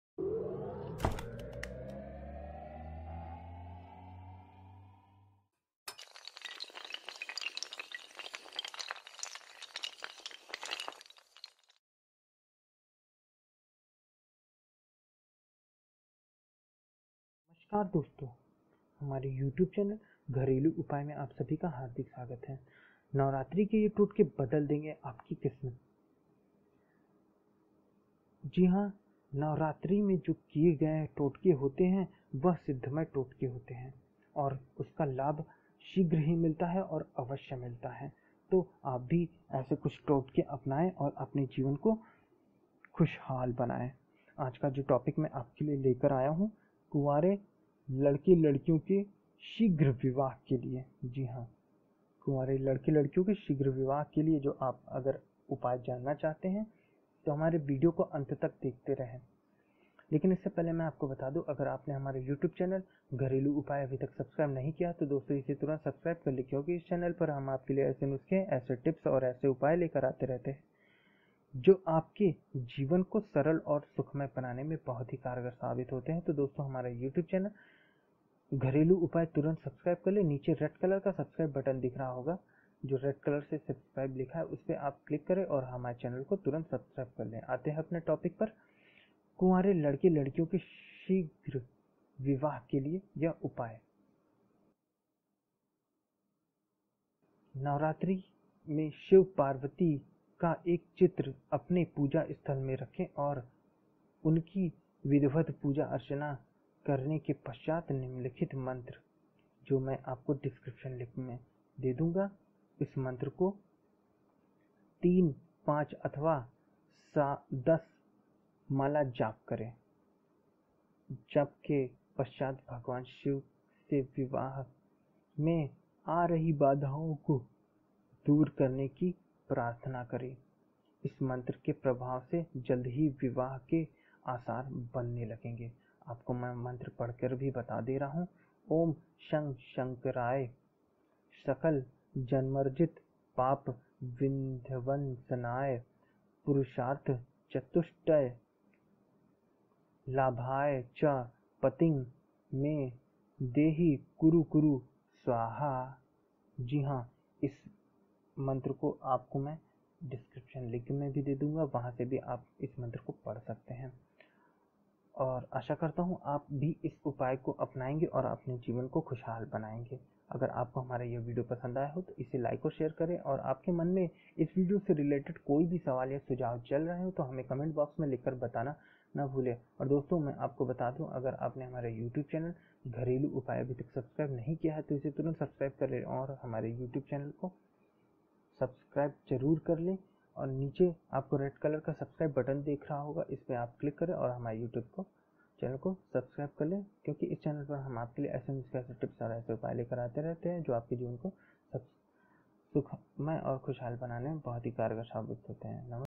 कुंवारे लड़के-लड़कियों के शीघ्र विवाह के लिए टोटका । नवरात्रि के ये टोटके बदल देंगे आपकी किस्मत
कुंवारे लड़के-लड़कियों के शीघ्र विवाह के लिए टोटका । नवरात्रि के ये टोटके बदल देंगे आपकी किस्मत | Gharelu Upay

कुंवारे लड़के-लड़कियों के शीघ्र विवाह के लिए उपाय

नवरात्रि में शिव-पार्वती का एक चित्र अपने पूजास्थल में रखें और उनकी विधिवत पूजा-अर्चना करने के पश्चात निम्नलिखित मंत्र का 3, 5 अथवा 10 माला जप करें। जप के पश्चात भगवान शिव से विवाह में आ रही बाधाओं को दूर करने की प्रार्थना करें। इस मंत्र के प्रभाव से जल्दी ही विवाह के आसार बन जाते हैं।

मंत्र
ऊँ शं शंकराय सकल-जन्मार्जित-पाप-विध्वंसनाय, पुरुषार्थ-चतुष्टय-लाभाय च पतिं मे देहि कुरु कुरु स्वाहा।।